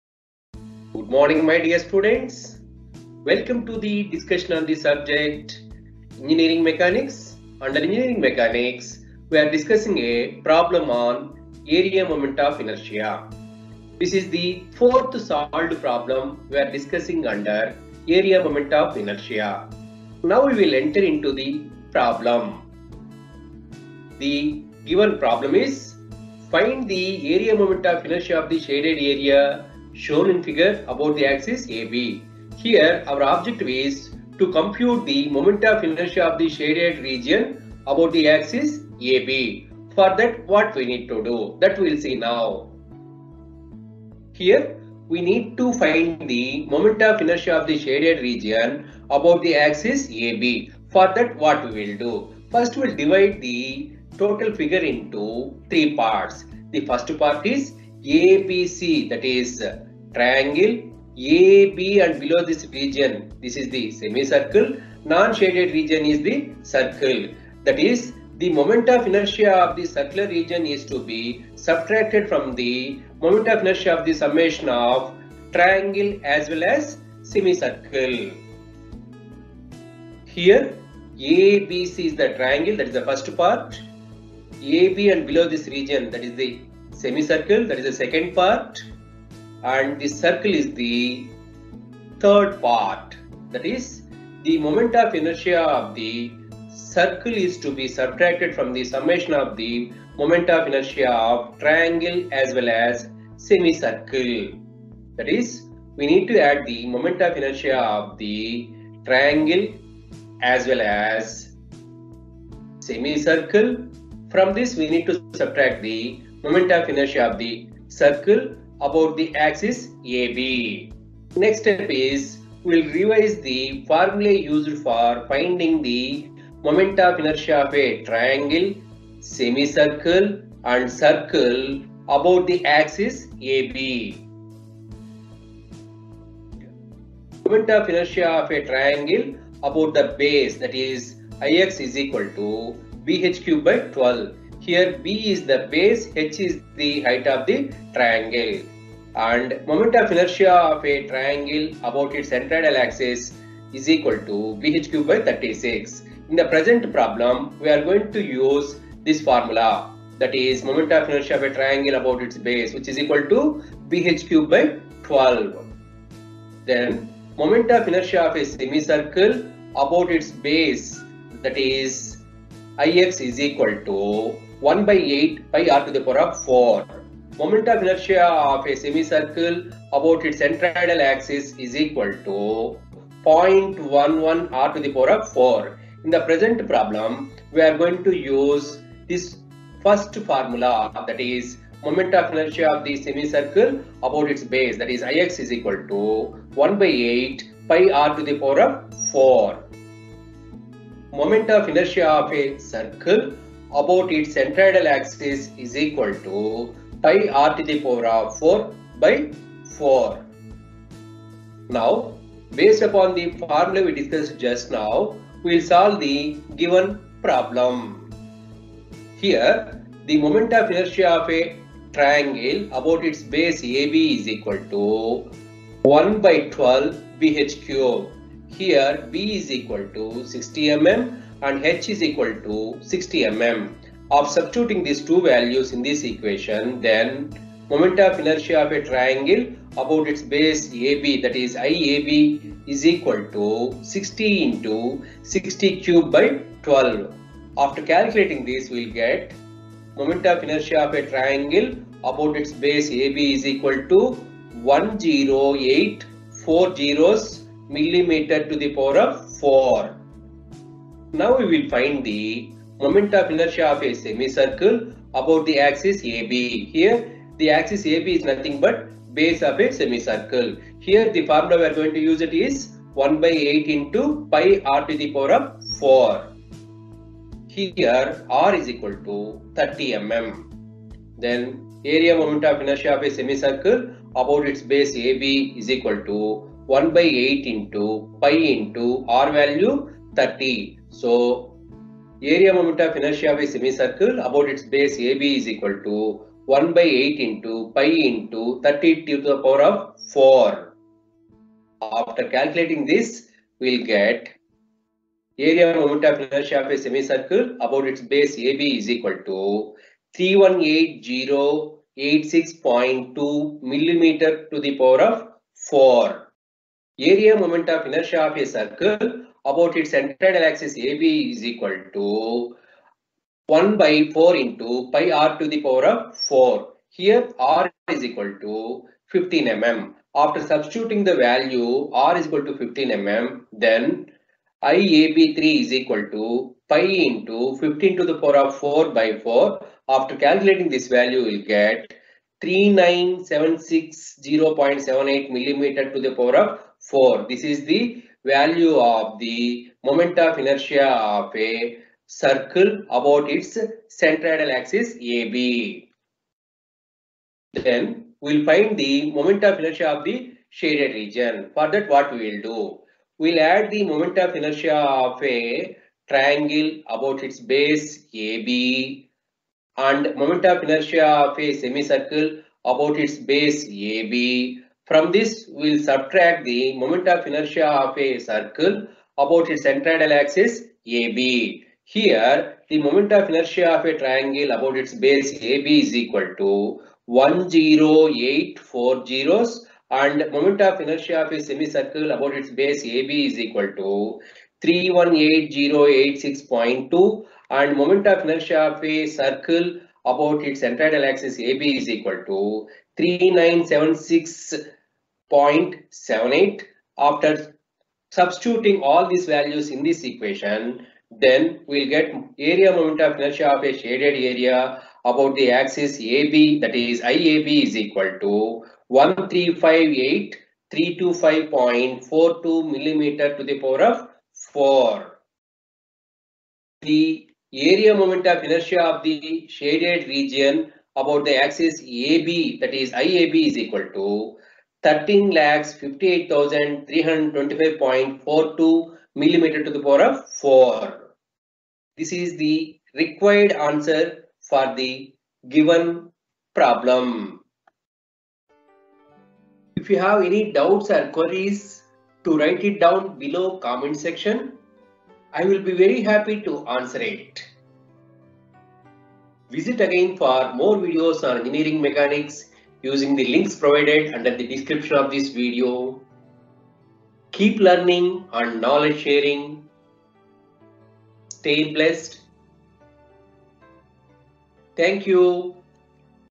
Area moment of inertia: problem 4
Hello everyone,
in this video  i  will explain, how you can solve the problem on moment of inertia of a composite section with circular hole. The composite section consists of triangle and semicircle with circular hole.
step 1: To find the centroid of the entire section
step 2: To find the moment of inertia of the section with respect to AB axis .